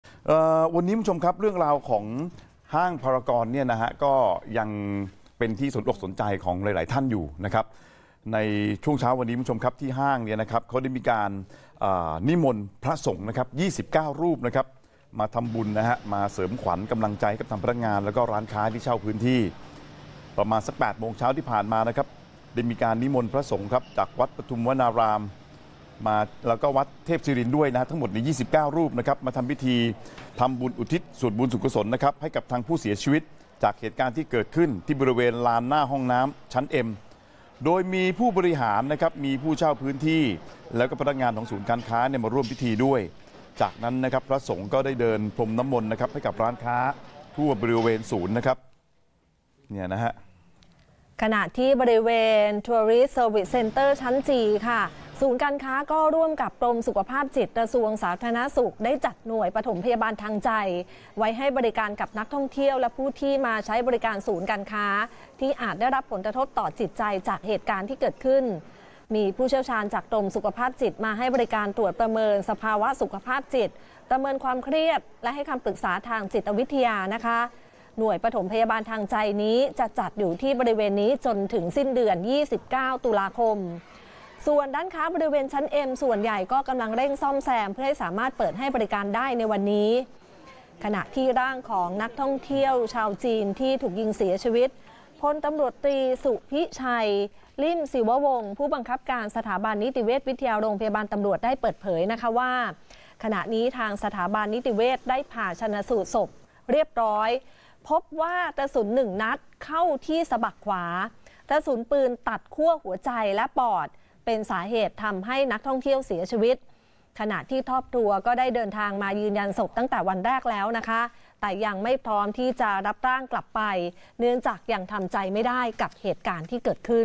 นิมนต์พระสงฆ์ 29 รูป สวดอุทิศส่วนกุศล 2 ศพถูกยิงในพารากอน พร้อมจัดบริการฟื้นฟูสุขภาพจิต
เช้านี้ที่พารากอน ทางผู้บริหารนิมนต์พระสงฆ์ 29 รูปมาทำบุญและสวดอุทิศส่วนกุศล เพื่อเสริมขวัญ กำลังใจให้กับพนักงาน-ร้านค้า พร้อมจัดบริการฟื้นฟูสุขภาพจิต

เมื่อประมาณ 8.00 น.ที่ผ่านมา นิมนต์พระสงฆ์จากวัดปทุมวนารามราชวรวิหาร และจากวัดเทพศิรินทร์ จำนวน 29 รูป มาทำพิธีทำบุญอุทิศส่วนกุศลให้กับผู้เสียชีวิตจากเหตุการณ์ที่เกิดขึ้น ที่บริเวณลานหน้าห้องน้ำชั้น M โดยมีผู้บริหาร ผู้เช่าพื้นที่ และพนักงานของศูนย์การค้ามาร่วมพิธี จากนั้นพระสงฆ์ได้เดินพรมน้ำมนต์ให้กับร้านค้าทั่วบริเวณศูนย์การค้า

ขณะที่บริเวณ Tourist Service Center ชั้น G ทางศูนย์การค้าฯ ได้ร่วมกับกรมสุขภาพจิต กระทรวงสาธารณสุข จัดหน่วยปฐมพยาบาลทางใจ ไว้ให้บริการกับนักท่องเที่ยว และผู้ที่มาใช้บริการศูนย์การค้าฯ ที่อาจได้รับผลกระทบต่อจิตใจจากเหตุการณ์ที่เกิดขึ้น โดยจะมีผู้เชี่ยวชาญจากกรมสุขภาพจิต มาให้บริการตรวจประเมินสภาวะสุขภาพจิต ประเมินความเครียด และให้คำปรึกษาทางจิตวิทยา โดยหน่วยปฐมพยาบาลทางใจจะจัดอยู่ที่บริเวณนี้ จนถึงวันที่ 29 ต.ค.นี้

ส่วนร้านค้าบริเวณชั้น M ส่วนใหญ่กำลังเร่งซ่อมแซม เพื่อให้สามารถเปิดให้บริการได้ในวันนี้ (5 ต.ค. 2566)

สำหรับร่างของนักท่องเที่ยวสาวชาวจีนที่โดนยิงเสียชีวิต พลตำรวจตรี สุพิไชย ลิ่มศิวะวงศ์ ผู้บังคับการสถาบันนิติเวชวิทยา โรงพยาบาลตำรวจ เปิดเผยว่า ขณะนี้ทางสถาบันนิติเวชฯ ได้ผ่าชันสูตรศพนักท่องเที่ยวหญิงชาวจีนเรียบร้อยแล้ว พบว่ากระสุน 1 นัดเข้าที่สะบักขวา กระสุนปืนตัดขั้วหัวใจและปอด เป็นสาเหตุให้เสียชีวิต ขณะที่ครอบครัวได้เดินทางมายืนยันศพตั้งวันแรกแล้ว แต่ยังไม่พร้อมจะรับร่างกลับ เนื่องจากยังทำใจไม่ได้กับเหตุการณ์ที่เกิดขึ้น

อ่านข่าวเพิ่มเติมได้ที่

เที่ยงวันทันเหตุการณ์ วันที่ 5 ตุลาคม 2566